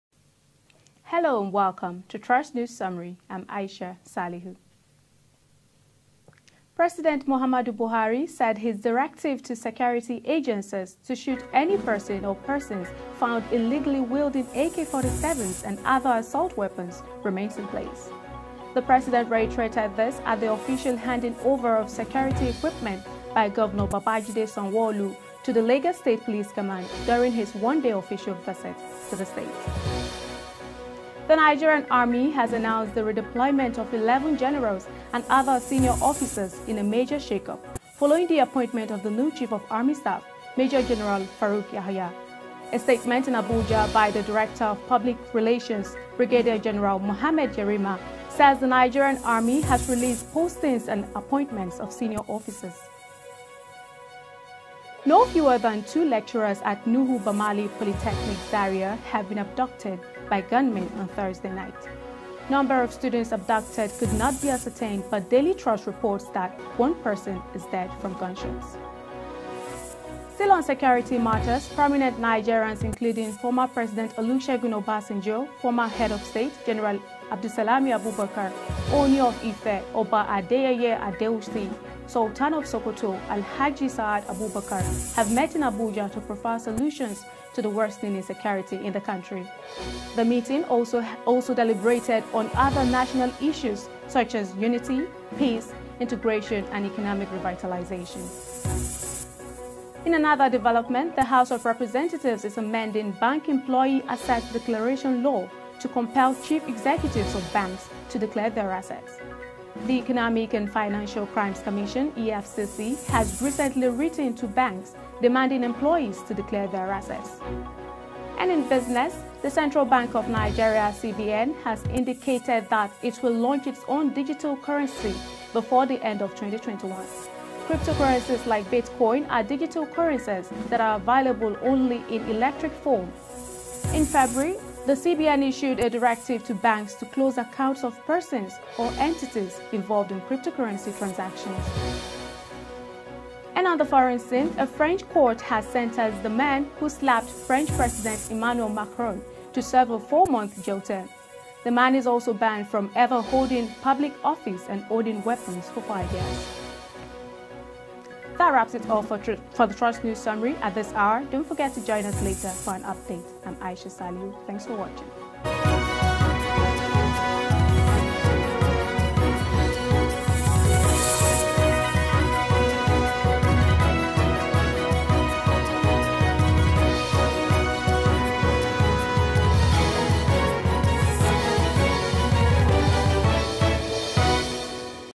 Trust News Summary 11th June: Buhari Asks Security Operatives To Shoot AK47 Carriers
Buhari reiterated his directive at the official handing over of security equipment to the Lagos State Police Command on Thursday. This and more on Trust News Summary.